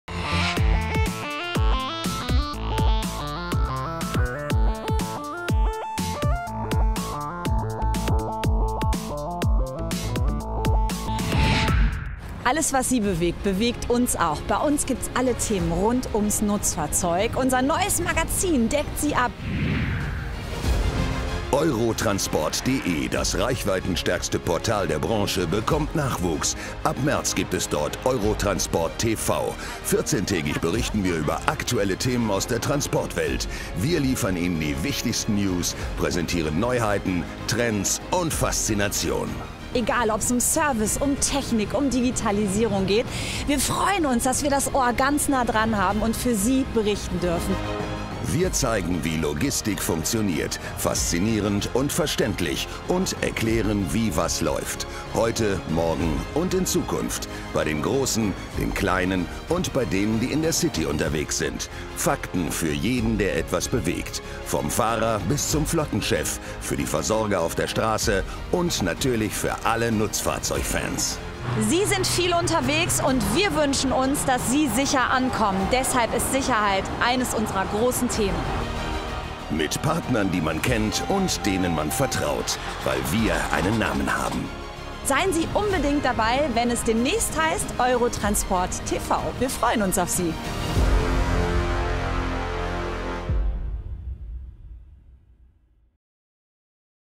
WebTV Magazin: eurotransportTV startet am 7. März 2019
ETM steigt ins WebTV ein: Am 7. März 2019 startet das neue Videoformat eurotransportTV. Damit richten wir uns künftig 14-täglich mit den wichtigsten News aus der Nutzfahrzeug- und Transportbranche an alle Berufskraftfahrer, Entscheider aus Transportunternehmen, Industrievertreter und Nutzfahrzeug-Begeisterte.

Für Nutzfahrzeug-Kompetenz und ein untrügliches Gespür für die besten Themen sorgen die Redakteure der Redaktionen FERNFAHRER und lastauto omnibus. Chefredakteur Thomas Rosenberger leitet neben den Redaktionen der beiden Fachmagazine auch die Redaktion von eurotransportTV und arbeitet eng mit fachkundigen TV-Reporter-Teams zusammen. Die Moderation des neuen Formats übernimmt Alexandra von Lingen.

Hier kommen echte Experten zu Wort

eurotransportTV informiert, berät und unterhält. Dazu gehört aber auch eine kritische Berichterstattung zu brennenden Themen wie Fahrermangel und Verkehrssicherheit. Mehr noch: eurotransportTV lässt hochkarätige Experten aus Spedition und Logistik sowie der Fahrzeug- und Zulieferindustrie zu den neuesten Trends der Branche zu Wort kommen. Darüber hinaus liefern unsere Experten wertvolle Insider-Informationen aus der Transportindustrie und außerdem einen kompakten Newsblock über alle Themenbereiche hinweg. Damit stehen wir für geballte Kompetenz und eine ausgewogene Themenmischung:

* detaillierte, fachkundige Fahrzeugtests
* Vorstellung neuer Lkw- und Transporter-Modelle
* packende Reportagen vor Ort
* spannende Features rund um die Truck Race-Szene
* kompakter Newsblock

Wo wird eurotransportTV ausgestrahlt?

eurotransportTV geht den direkten Weg zum Zuschauer – die gesamte Sendung sowie einzelne Clips laufen über:

* Online-Portal eurotransport.de
* YouTube-Kanäle von eurotransport.de und FERNFAHRER
* Facebook-Auftritte von FERNFAHRER, lastauto omnibus und eurotransport.de